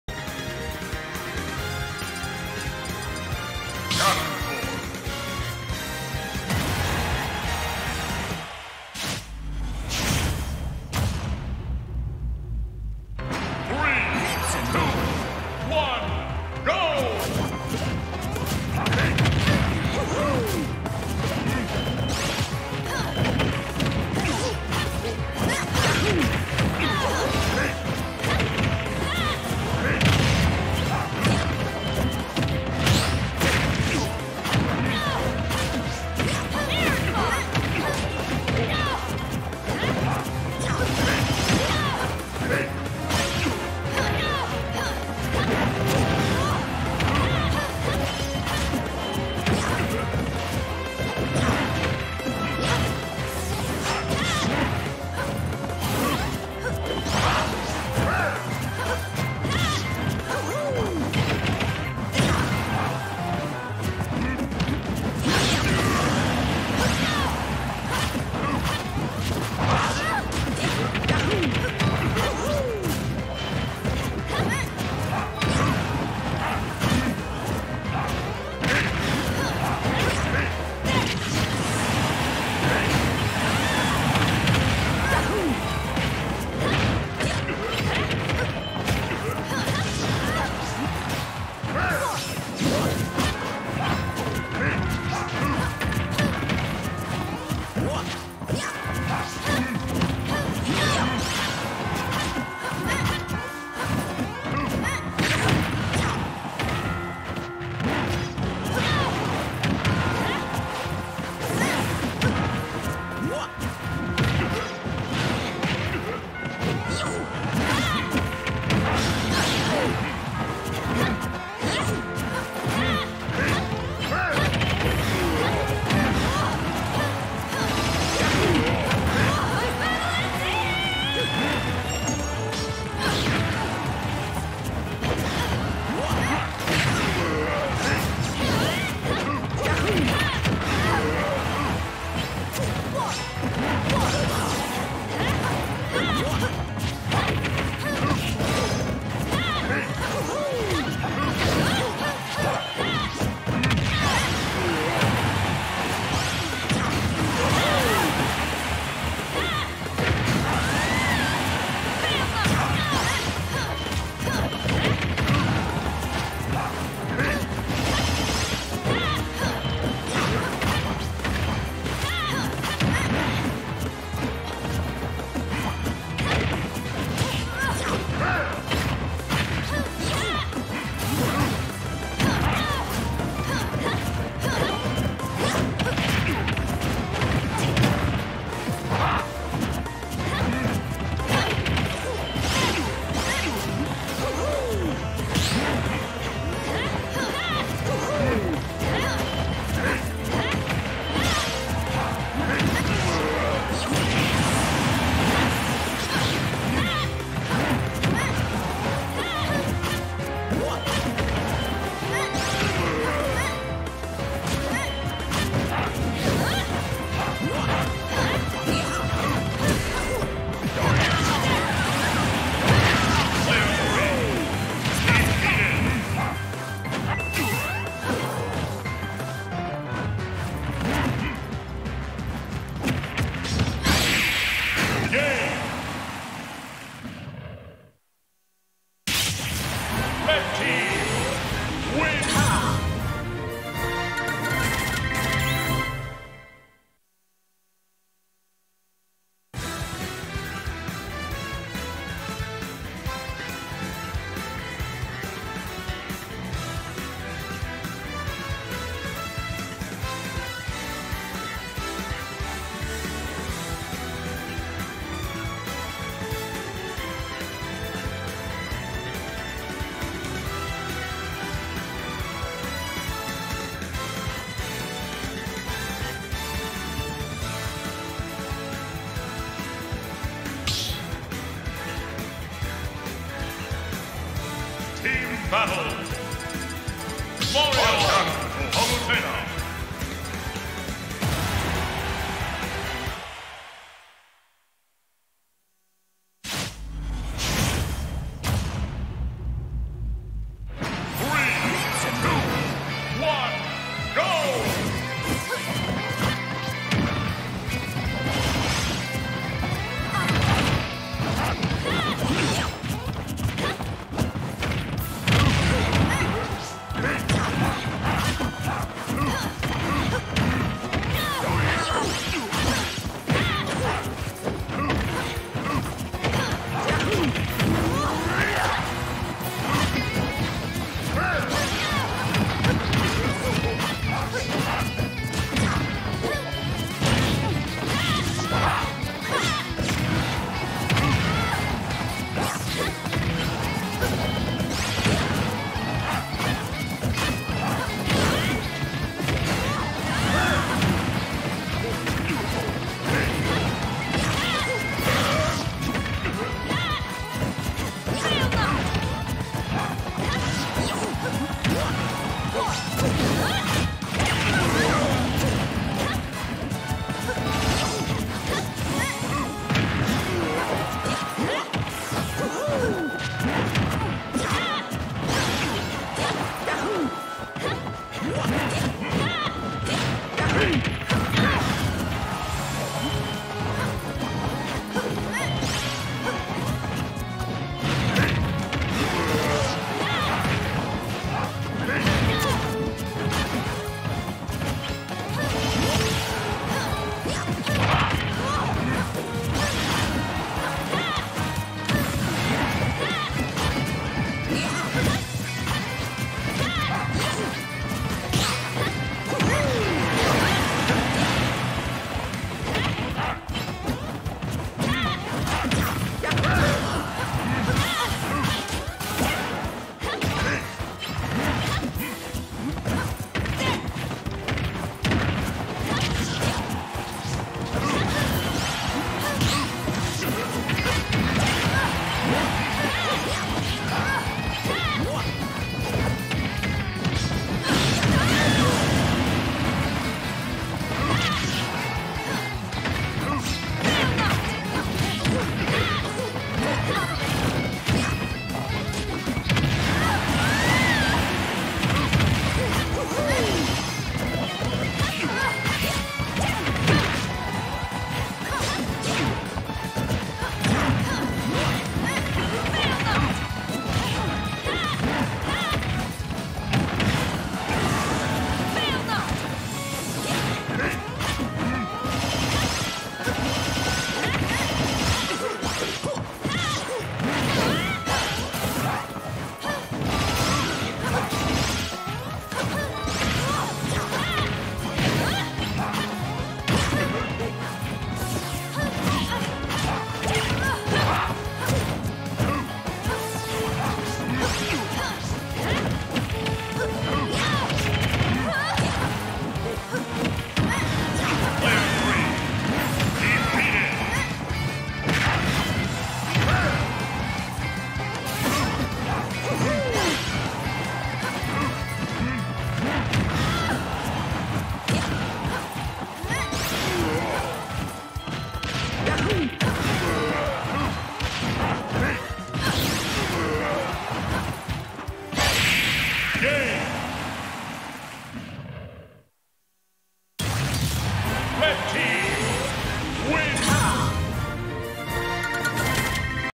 Frame by Frame | Winners R3 - Doubles | Blender & Ssabado vs. Gianic & Doelo
Frame by Frame 2022 Puerto Rico's west premier fighting game event hosted on June 4 at the UPRM Centro de estudiantes in Mayagüez, Puerto Rico.